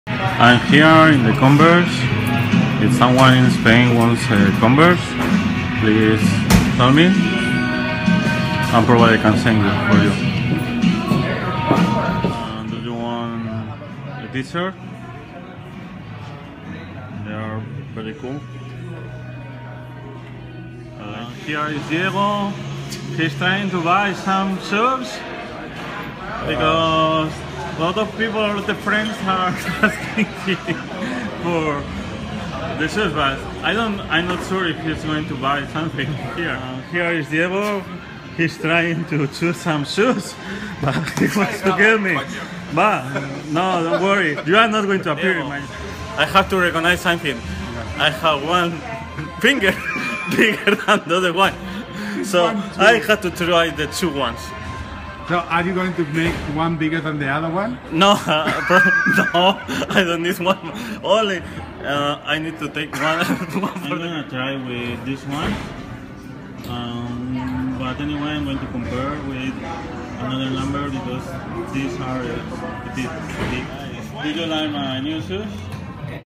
Vblog 13: Fun in the converse store at San Francisco
Buying shoes in Converse Store